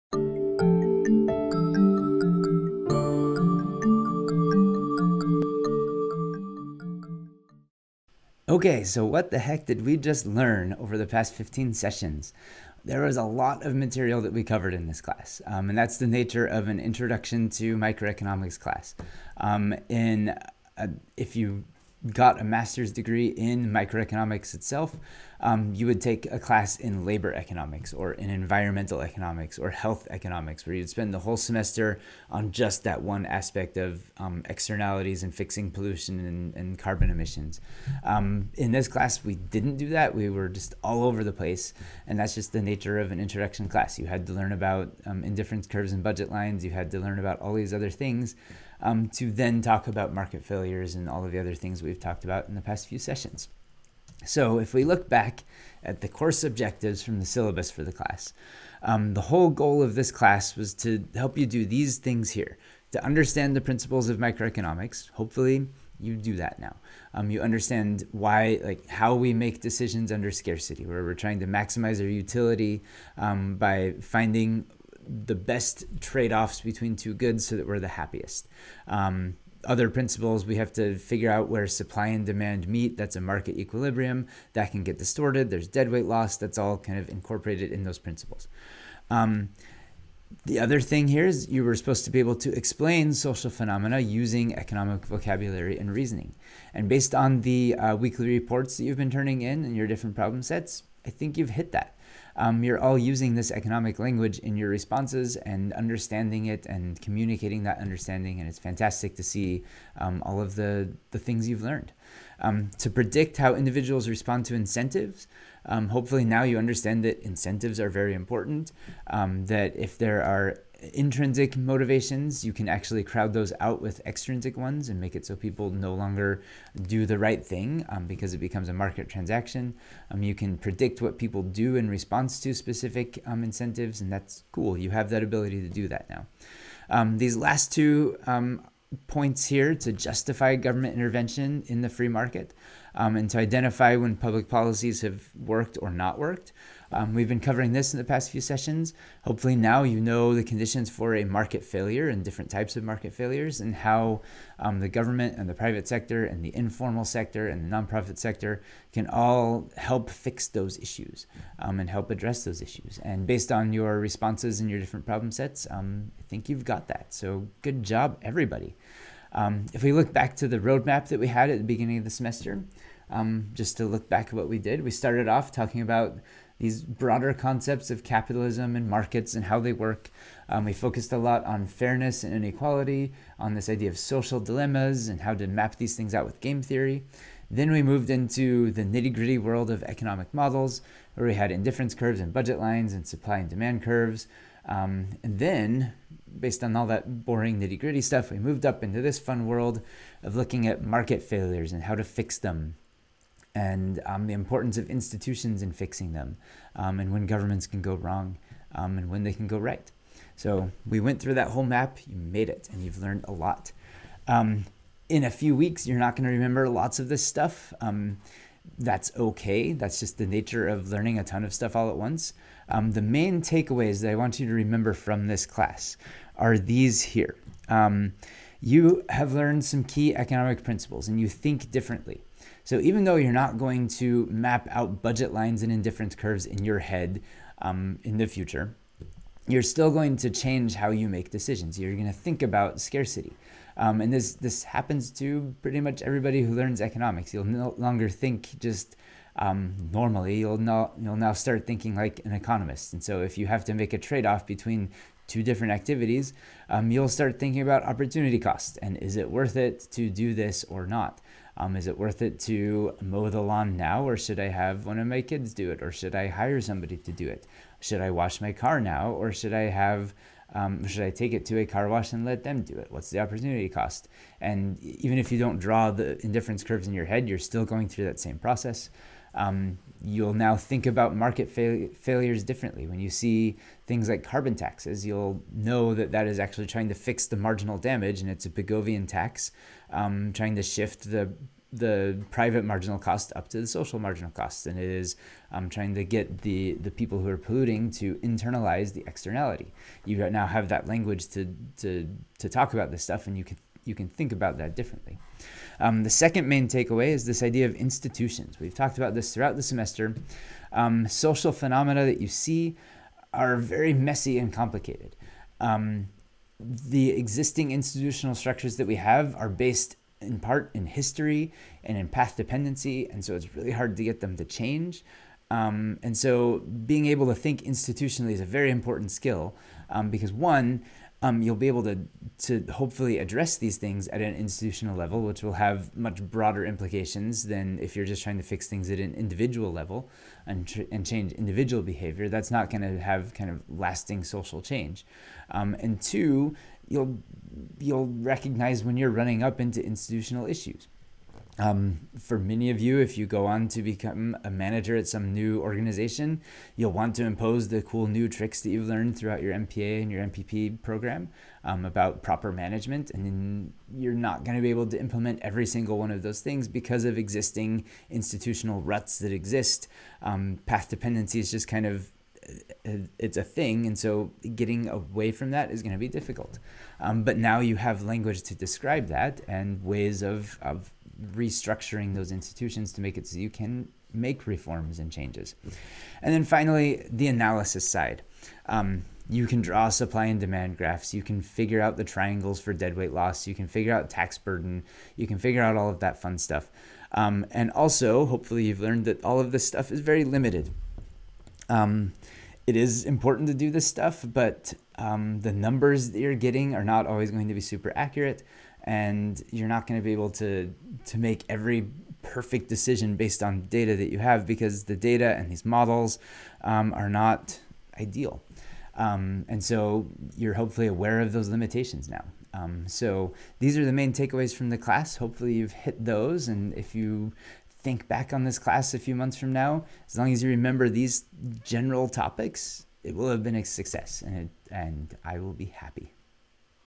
PMAP 8141 • (15) Markets, public policy, & public administration: (1) What the heck did we learn?!
Overview of everything we just covered in the class

Access the slides and other materials at the course